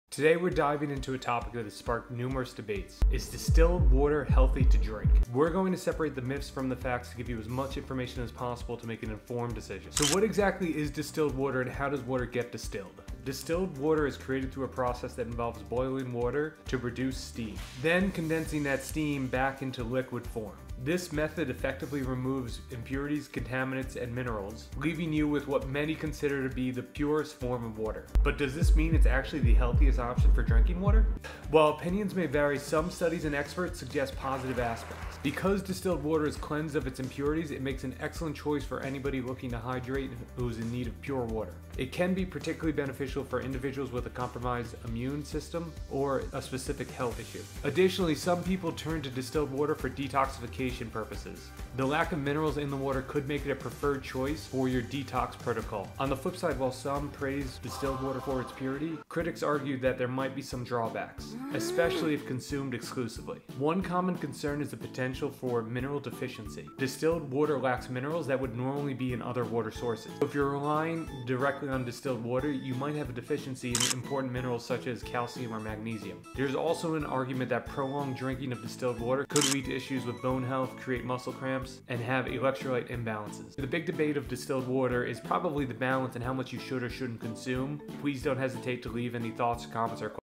Is Distilled Water Safe to Drink? (How To Make Distilled Water)
Today, we're diving into a topic that has sparked numerous debates: Is distilled water healthy to drink? We'll separate the myths from the facts and give you the information you need to make an informed decision."

Now, let's delve deeper into the distillation process. Distilled water is created through a meticulous process that involves boiling water to produce steam, then condensing that steam back into liquid form. This method effectively removes impurities, contaminants, and minerals, leaving you with what many consider to be the purest form of water."
But does this mean it's the healthiest option for regular drinking? Let's explore further.
While opinions may vary, some studies and experts suggest several positive aspects.
First and foremost because distilled water is devoid of impurities and contaminants, making it an excellent choice for those looking to hydrate with pure water. It can be particularly beneficial for individuals with compromised immune systems or specific health conditions."
Additionally, some people turn to distilled water for detoxification purposes. The absence of minerals and contaminants could make it a preferred choice for certain detox protocols
While some praise its purity, critics argue that it may come with drawbacks, especially if consumed exclusively."
One commonly cited concern is the potential for mineral deficiencies. Distilled water lacks minerals that are naturally present in other water sources. Some argue that relying solely on distilled water could lead to a deficiency in essential minerals like calcium, magnesium, and potassium."
Critics argue that prolonged consumption of exclusively distilled water may contribute to issues such as electrolyte imbalances, muscle cramps, and even potential impacts on bone health
So, the debate here revolves around striking a balance. While distilled water can be a great choice for purity, it might not be the best sole source of hydration for everyone